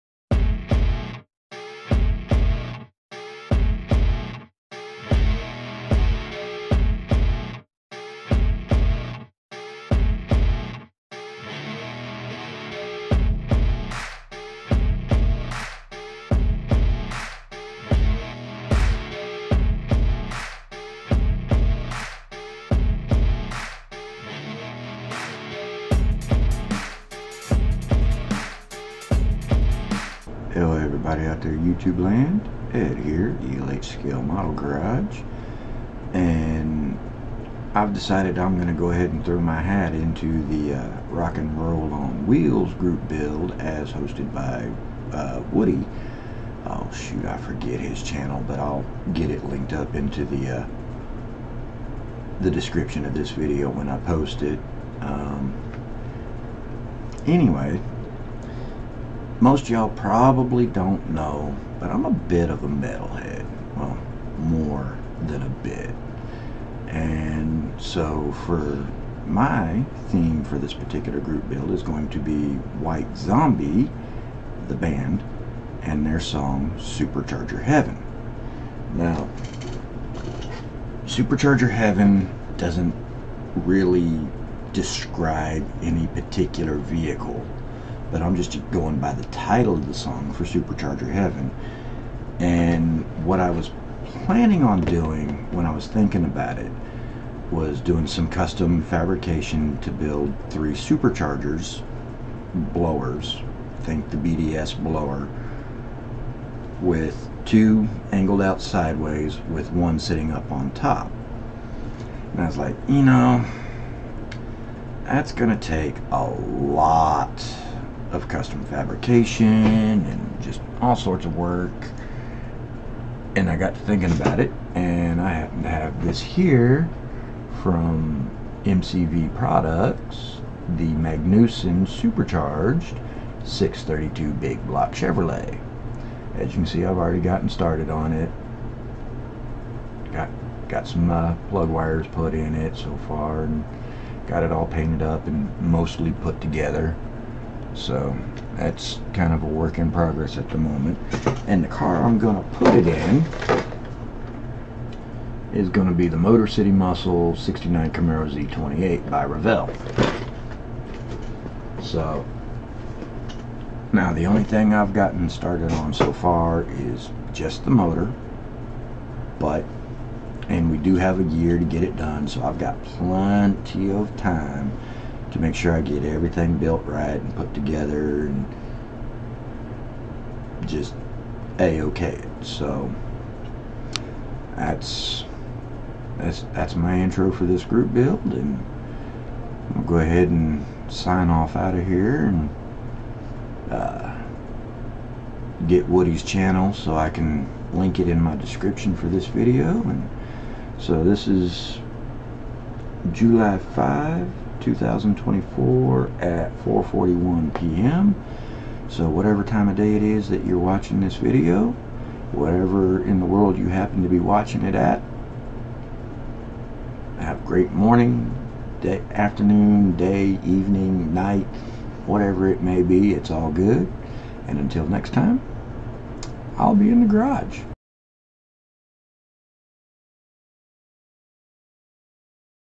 Rock&Roll on Wheels GB intro
This is my intro video for the rock&rollonwheels GB as hosted by Woody over @WoodysScaleModelGarage In case you're wondering about the blue paint on my arm and hand, that's for a project I'm working on for my model club. I'll do a video on that later today.